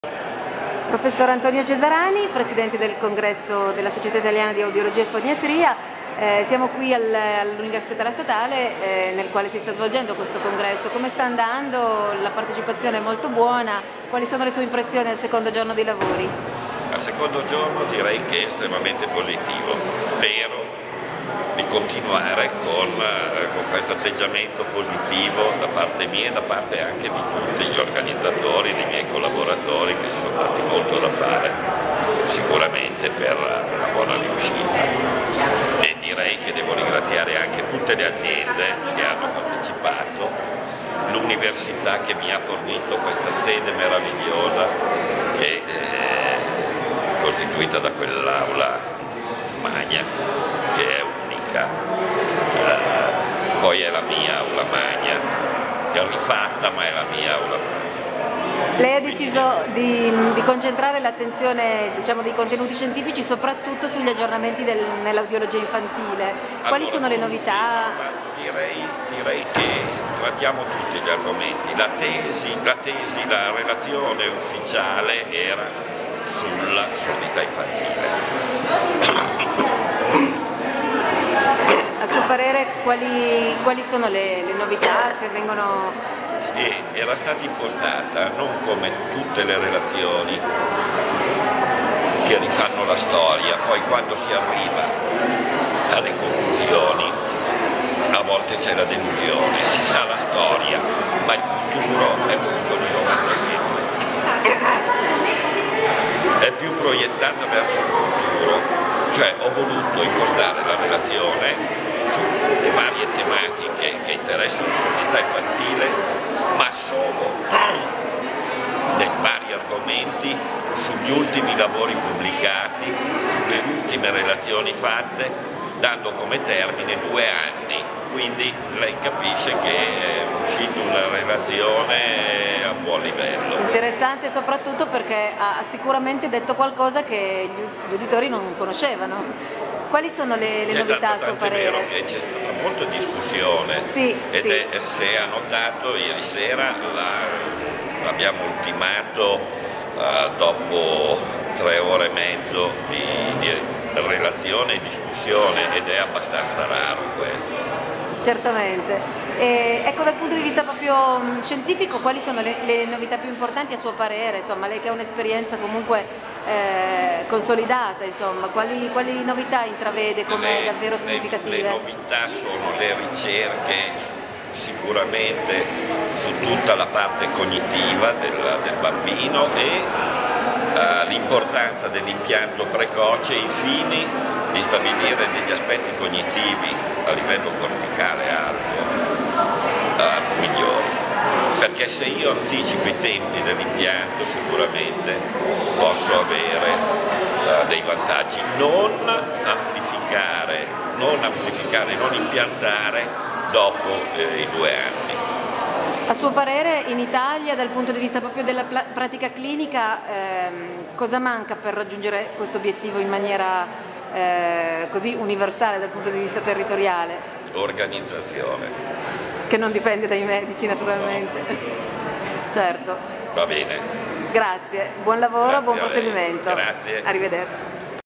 35° SIAF Milano 2015, nel segno del presidente Antonio Cesarani
La relazione ufficiale sull'audiologia infantile con le ultime ricerche in materia, il congresso ospitato nella storica Aula Magna: giorni fitti e ricchi di emozione per lo specialista dell'Università “Ca' Granda”.